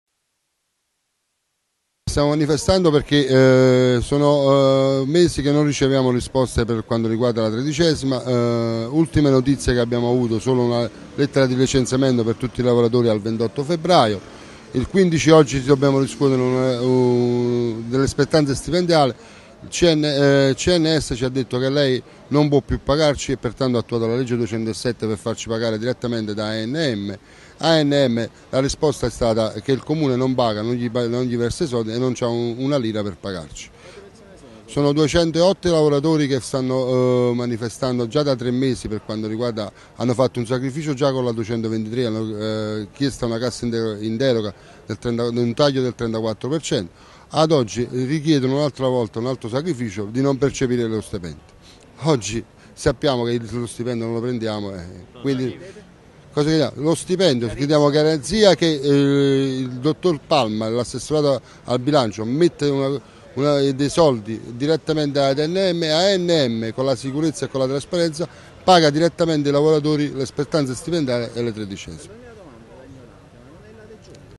Napoli - Anm, protesta azienda pulizie (16.01.13)
- Alcuni dipendenti di società partecipate del Comune in servizio per l'Anm (azienda del trasporto su ruote cittadina) hanno occupato la rimessa degli autobus "Carlo III" per denunciare i mancati pagamenti e il futuro licenziamento. Alcuni dei manifestanti sono saliti su un cornicione e hanno minacciato di gettarsi giù. (16.01.13)